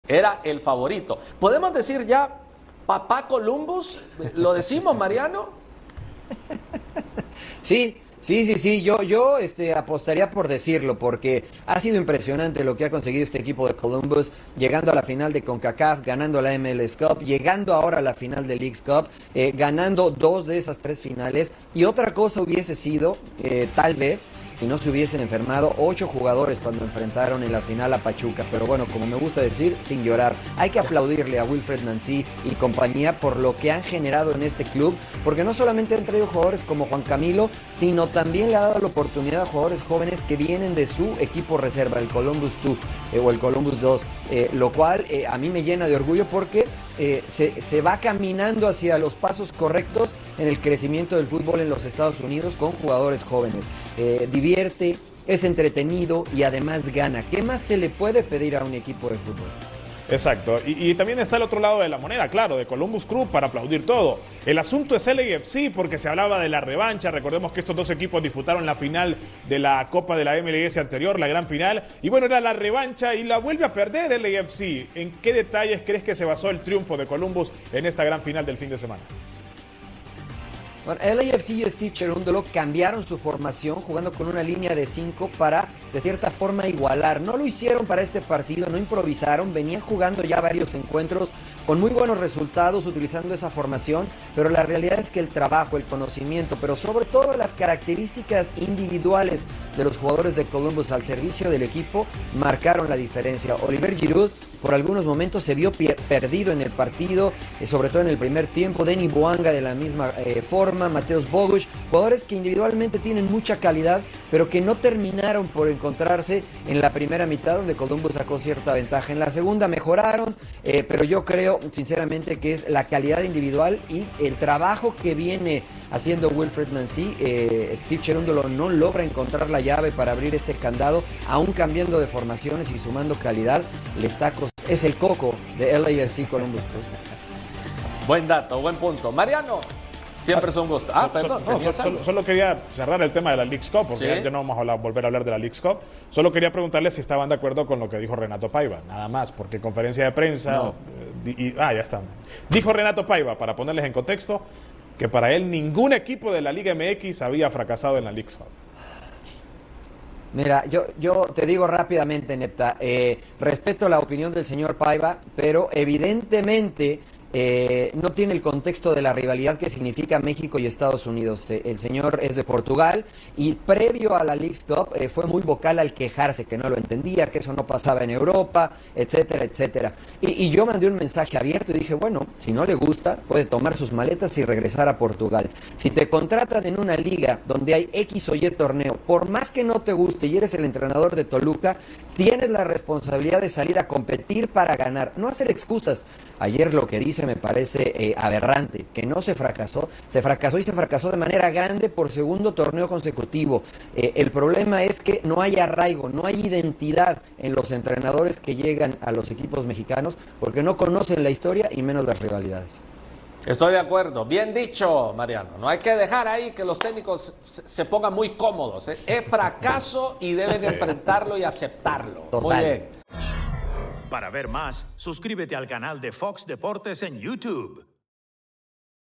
🥳🏆 ¡COLUMBUS CREW SE LLEVA LA LEAGUES CUP! | GolXGol
¡COLUMBUS CREW SE LLEVA LA LEAGUES CUP! 🥳🏆 Mariano Trujillo analiza cómo Columbus Crew venció a LAFC en la final de la Leagues Cup y por qué ningún equipo de la Liga MX llegó a la final.